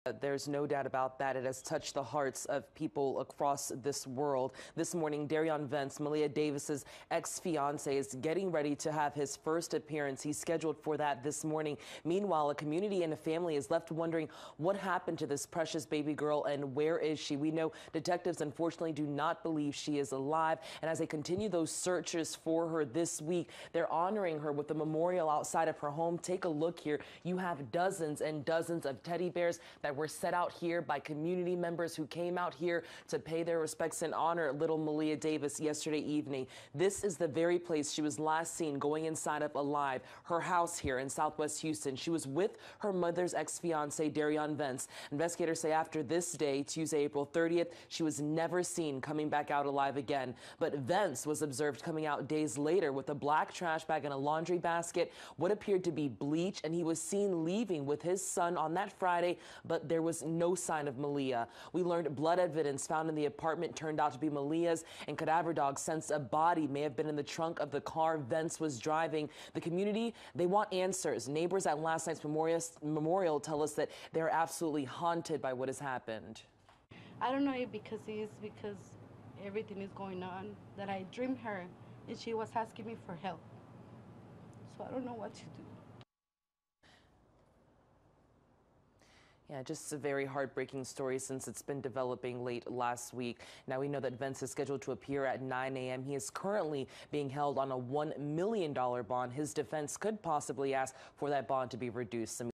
Maleah Davis: Derion Vence in custody and charged with evidence tampering
Derion Vence is facing a charge of evidence tampering, namely a corpse, in the 4-year-old's disappearance